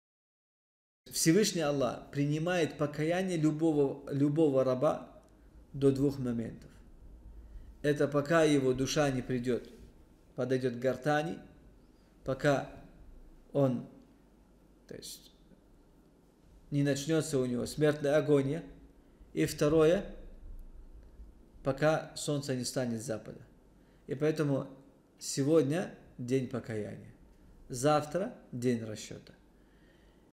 Завтра день расчета!
Сегодня день покаяния завтра день расчета -- шей Сулейман Хайруллаев

г. Киев, ул. Галагановская, 6